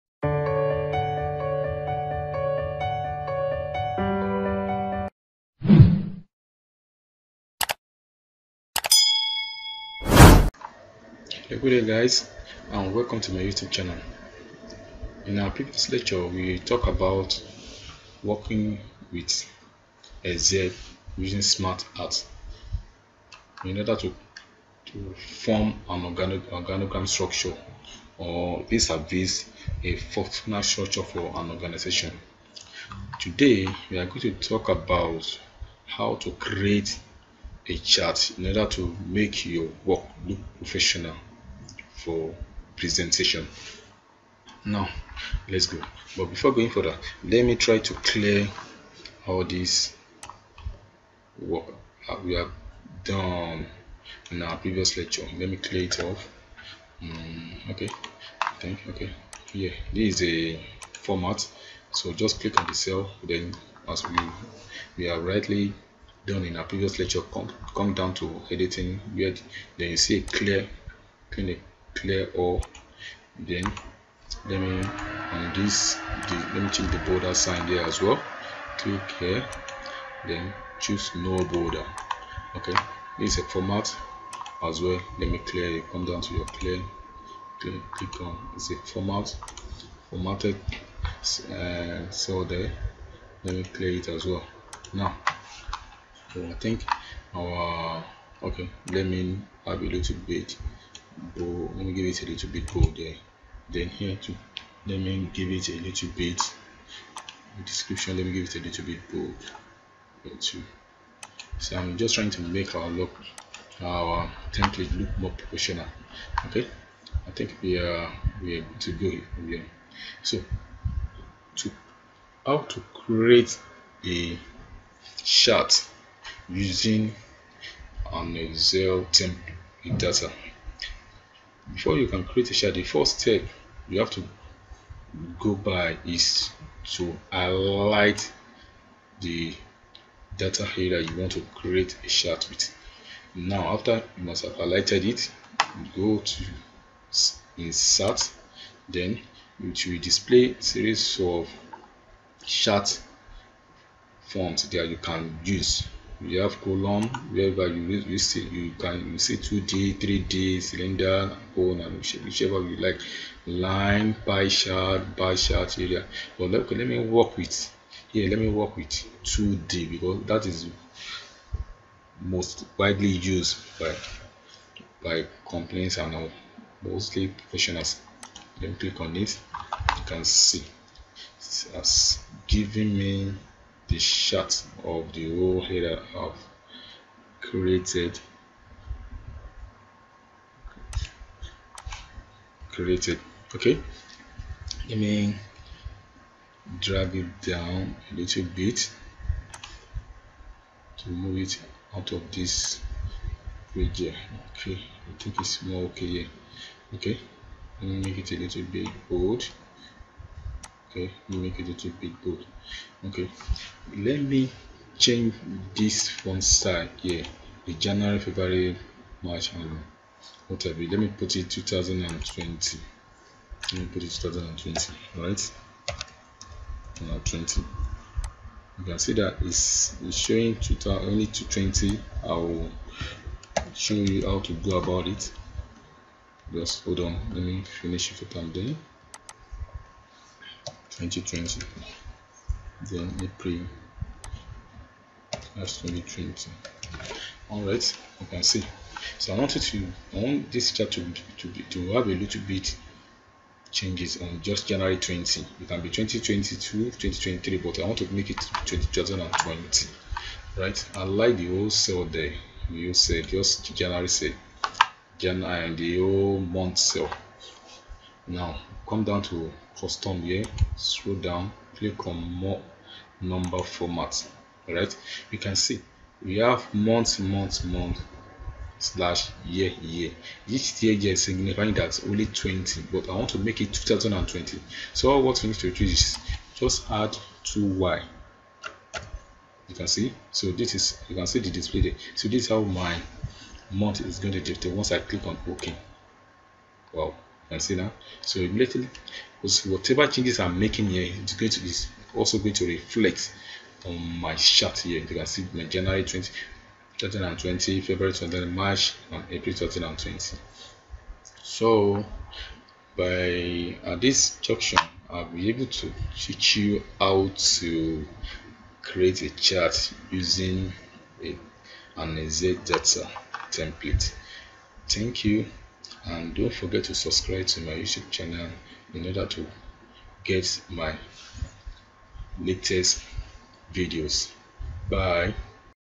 How To Create A Chart in An Excel
Learn: The Quick & easy way on How To Create A Chart in An Excel

In this video, you will learn:
1. how to create and read a chart
2. how to create Graphs in Excel
A simple chart in Excel can say more than a sheet full of numbers. As you'll see, creating charts is very easy.